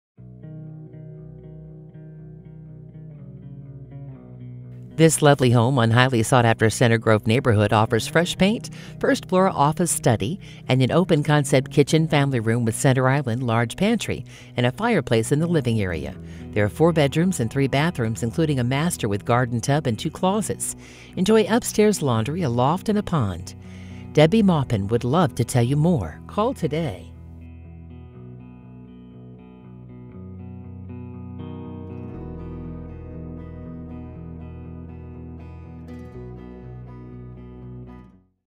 431 Wakefield Court Greenwood, IN | MLS# 21889638
431 Wakefield Court, Greenwood, IN

Lovely home in a highly sought-after Center Grove neighborhood. Freshly painted walls throughout the home. Office/study on the first floor. SS appliances stay. Open-concept kitchen/family room with center aisle, large pantry, and a fireplace in the living area. The laundry room is conveniently locat